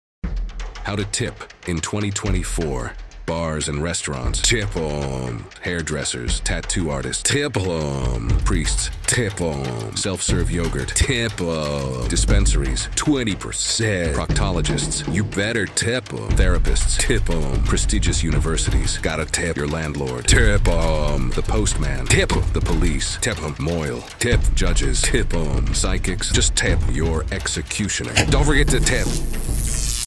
Ultimate Tipping Guide 2024: Who, When & How Much? | Expert Tips & Etiquette!
Welcome to the ultimate tipping guide for 2024! Unsure about tipping etiquette? We've got you covered. In this comprehensive video, we break down everything you need to know about tipping, including who to tip, when to tip, and how much is appropriate. Whether you're dining out, traveling, or receiving services, understanding tipping customs is essential. Join us as we navigate through various scenarios, providing expert insights and practical tips to ensure you tip like a pro. Don't miss out on maximizing your gratitude and appreciation for exceptional service!